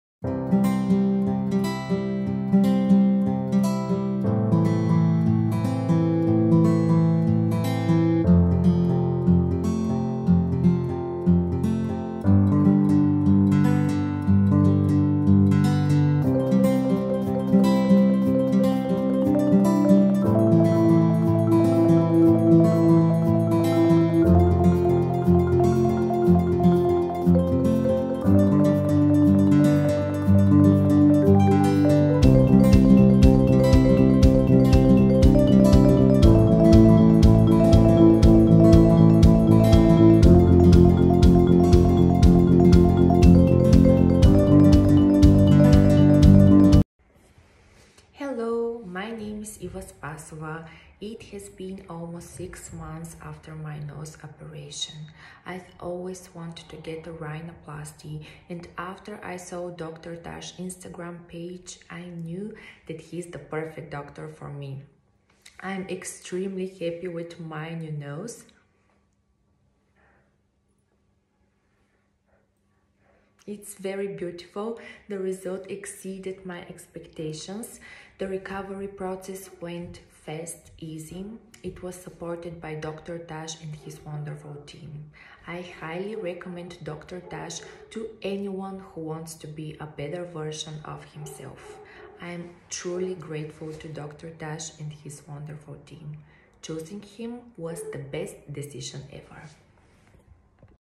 The best nose job with before and after
All hard work, ambition, and passion are just for that moment which is really wordless 🙏🏻 All we TAS team are grateful for the words of our lovely patient 🤗

Prof.Dr.Suleyman Tas
Plastic Reconstructive and Aesthetic Surgeon